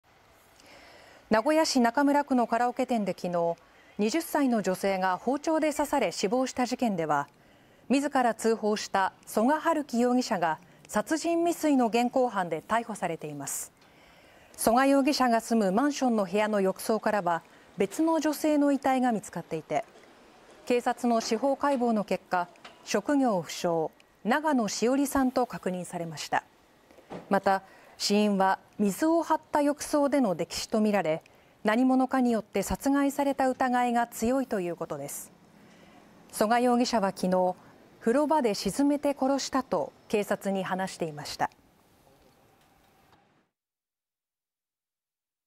カラオケ店で女性が刺され死亡した事件 “その後発見の遺体”は30歳女性で溺死 逮捕の男が住む部屋の浴槽に
26日、名古屋駅近くのカラオケ店で女性が刺され死亡した事件で、逮捕された男が住むマンションで死亡していた女性は、住人とみられる30歳の女性で、死因は溺死とみられることがわかりました。

名古屋市中村区のカラオケ店で26日、20歳の女性が包丁で刺され死亡した事件では、自ら通報した曽我春暉容疑者(25)が殺人未遂の現行犯で逮捕されています。

曽我容疑者が住むマンションの部屋の浴槽からは、別の女性の遺体が見つかっていて、警察の司法解剖の結果、職業不詳・長野汐里さん(30)と確認されました。

死因は、水を張った浴槽での溺死とみられ、何者かによって殺害された疑いが強いということです。

曽我容疑者は26日、「風呂場で沈めて殺した」と警察に話していました。

2人は事件前日の夕方、一緒にマンションを出入りする様子が近くの防犯カメラに映っていたということで、警察が事件の経緯を詳しく調べています。

2023年12月27日放送